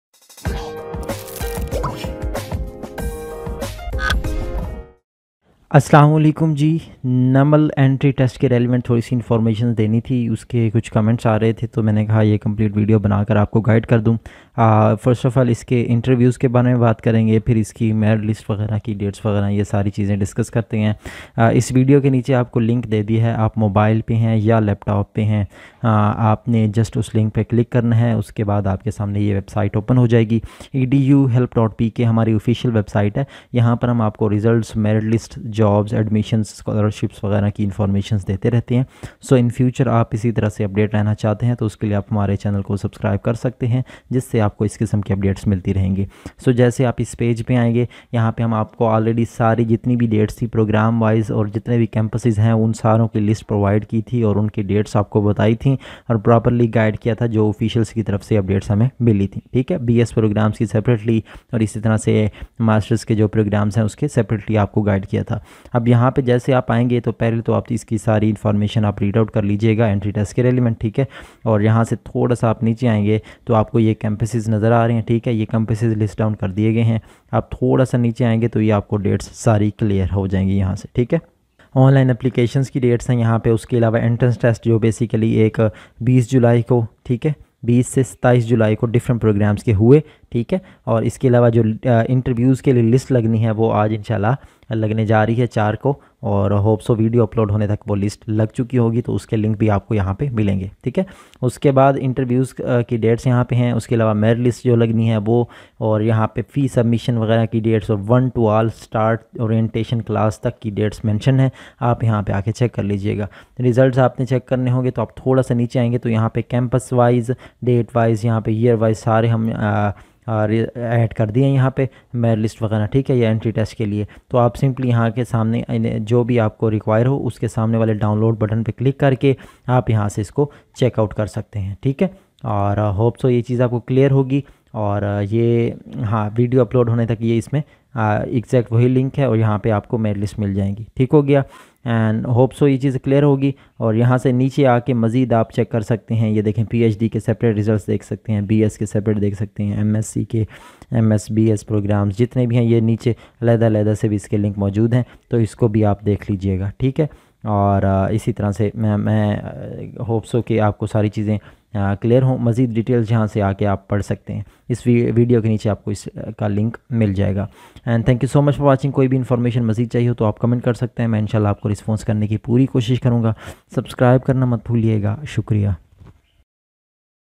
NUML Entry Test Result 2022 Announced Interview List
NUML Entry Test Result 2022 Announced Interview List. NUML Entry Test Result 2022 and NUML Merit List 2022 BS, MS, MSc, MCS check online from this page. The National Modern Language University The results of the GAT Entry test can be viewed here. Participants in the GAT who are interested in attending NUML University.